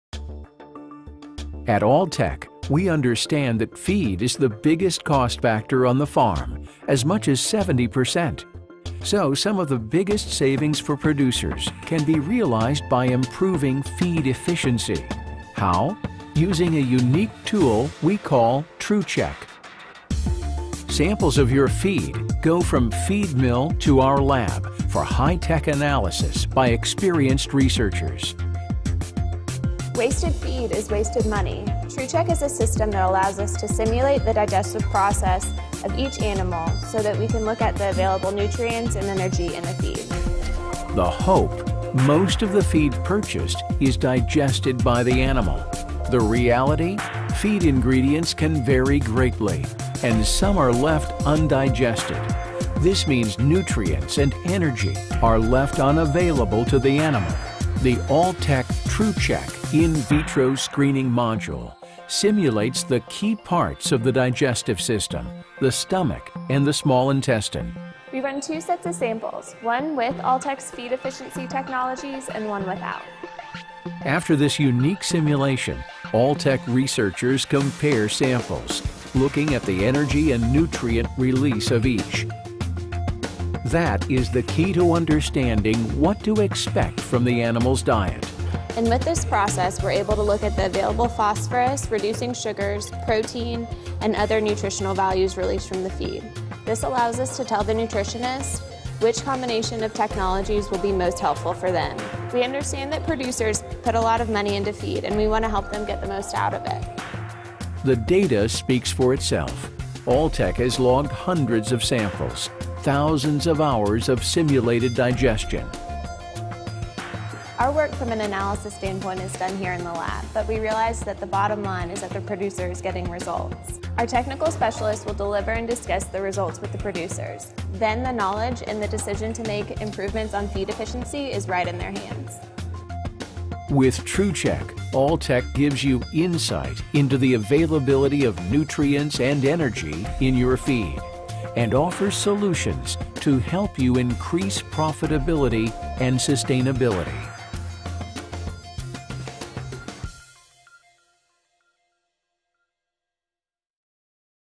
True Check for Pigs
With True Check, Alltech gives you insight into the availability of nutrients and energy in your feed.